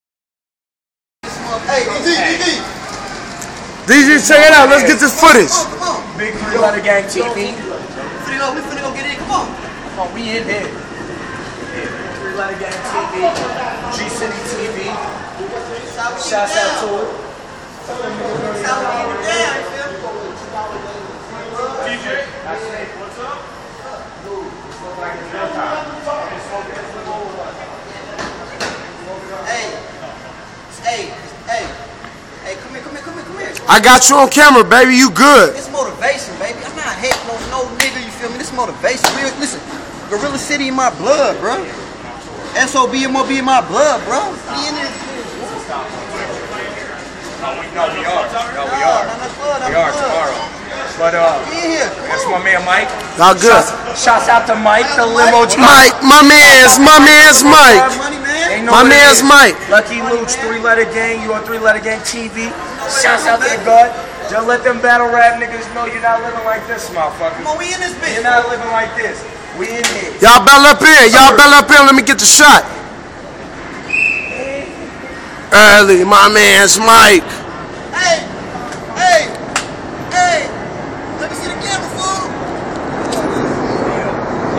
Garilla City landlordz (Gctv) 40 hammer thang...
This video was uploaded from an Android phone.in Vegas on the Battle America tour...live raw footage